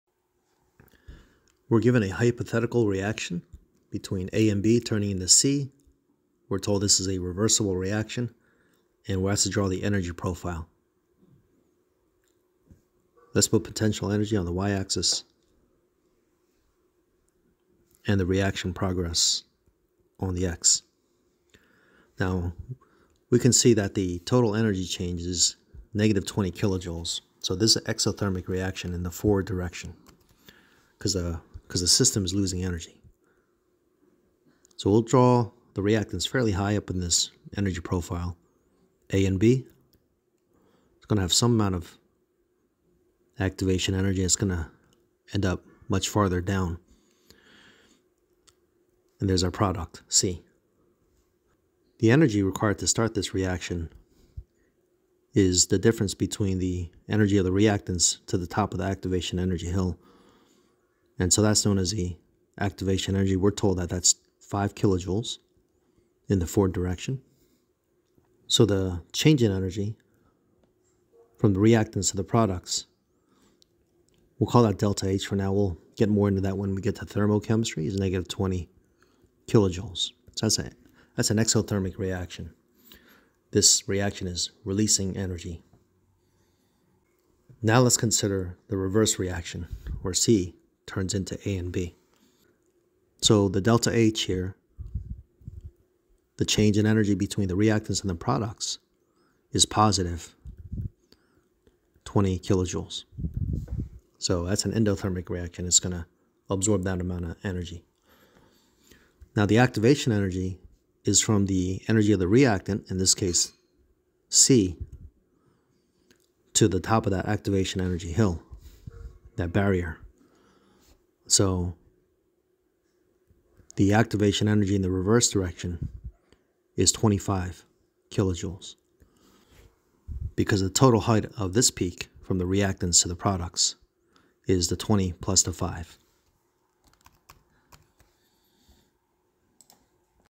Determining Activation Energy of a Reverse Reaction | Energy Profiles, Reaction Coordinate Diagram
Chapter 18, Problem 85:
For the reaction A = B C, the activation energy of the forward reaction is 5 kJ, and the total energy change is -20 kJ. What is the activation energy of the reverse reaction?